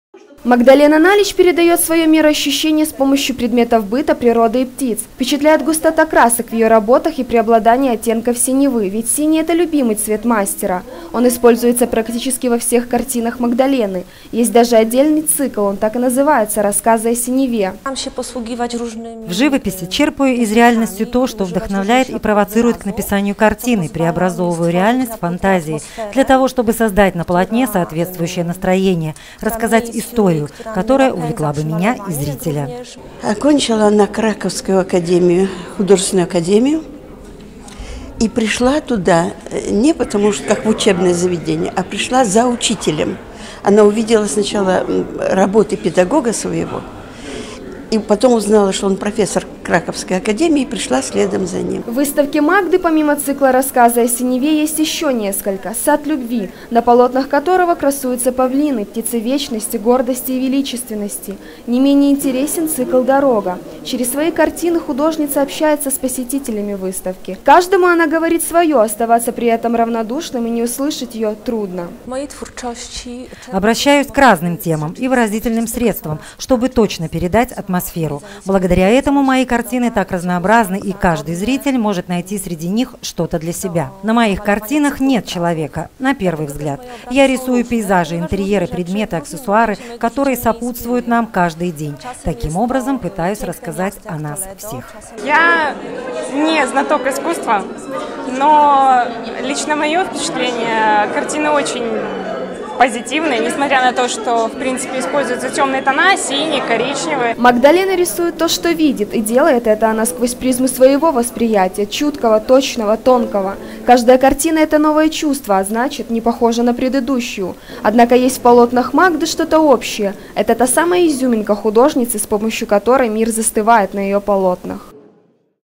2013-08-08 г. Брест Телекомпания "Буг-ТВ". Выставка польской художницы Магдалены Наленч.
Журналист К. Лесник.  Оператор В. Холявко
Выставка в Художественном музее польской художницы Магдалены Наленч.
В Брестском художественном музее состоялся вернисаж  - яркий и необычный. Свои работы на суд зрителей представила польская художница Магдалена Налиеч. Сухое слово работы, не совсем правильное обозначение того настроения, которое испытывают любители и знатоки живописи, глядя на полотна  талантливого мастера.
Её картины -- это окружающая нас природа и предметы быта, обыкновенные в своей необыкновенности, яркие в своей туманности, непосредственные в своей непонятности и какие-то очень близкие по сути всем людям.